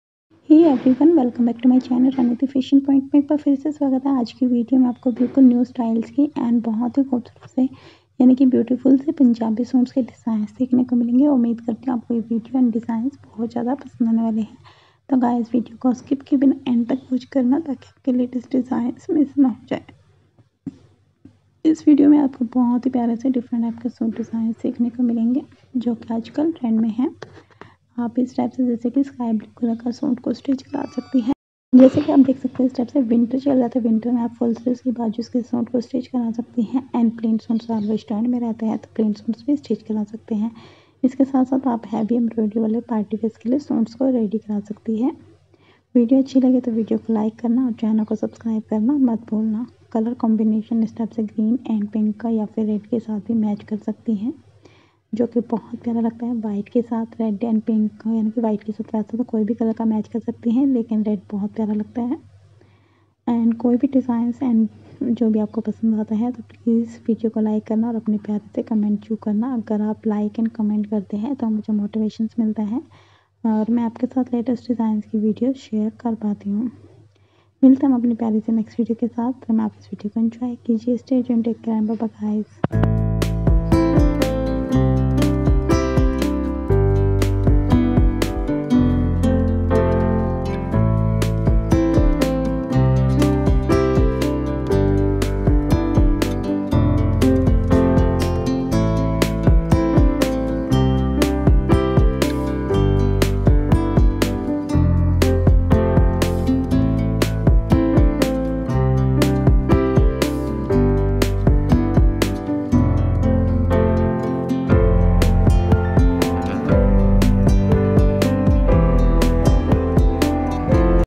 New Style Punjabi Suit Design//Beautiful suits//Traditional punjabi suit
aaj ki video m apko bhut ho khoobsurat traditional punjabi suit ke design dekhne ko milnge umeed h video helpful lge

Thanks❤🌹🙏